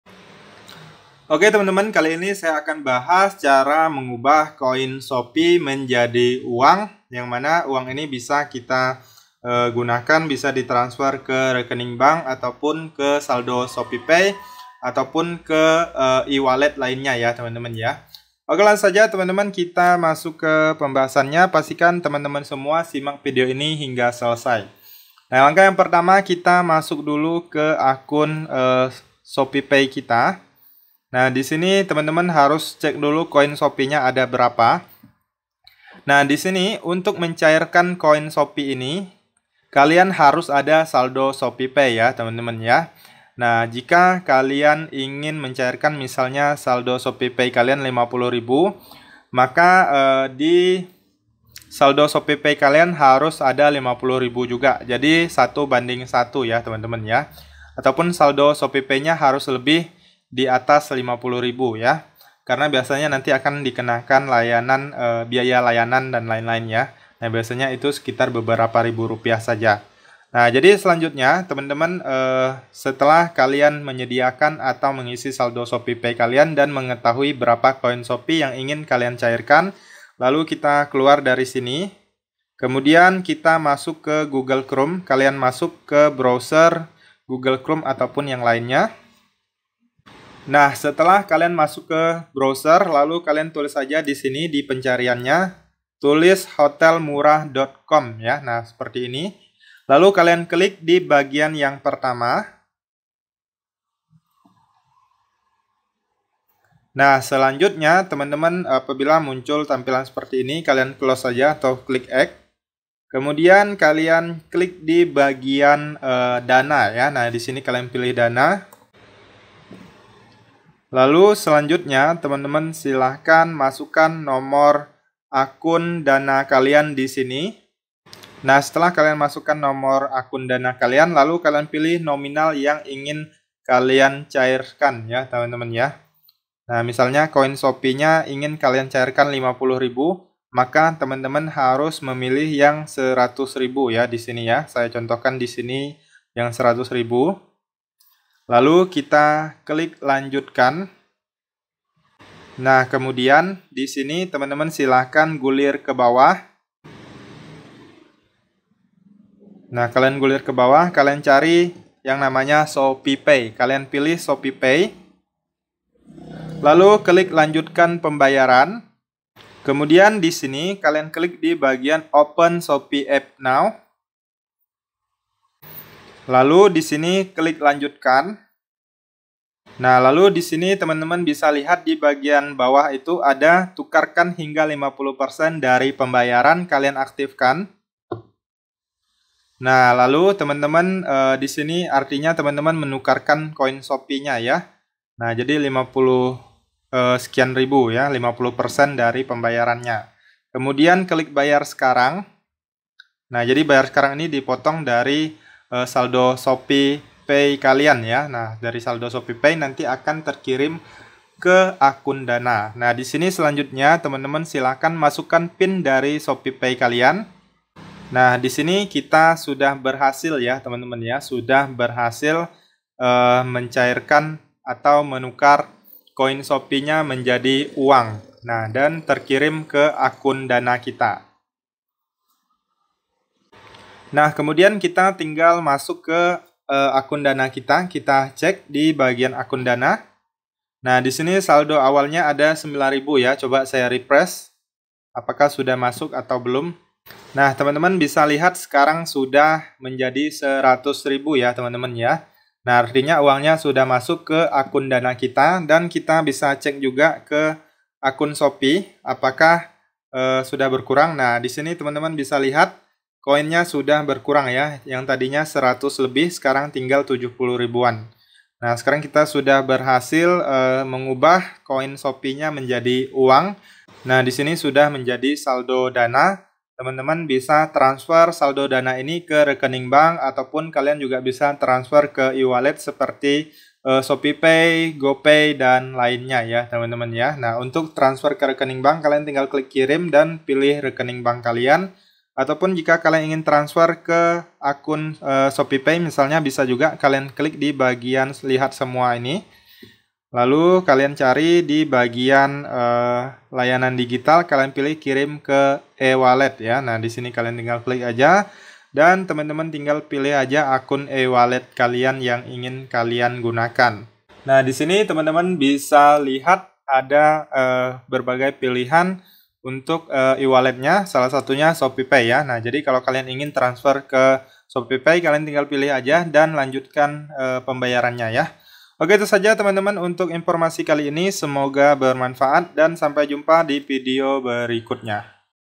Cara Tukar Koin Shopee Jadi Uang Terbaru 2024
Cara tukar koin shopee jadi uang.
atau cara tukar koin shopee jadi saldo shopeepay, bisa juga mengubah koin shopee menjadi saldo dana, gopay, ovo, dll.
semoga bermanfaat.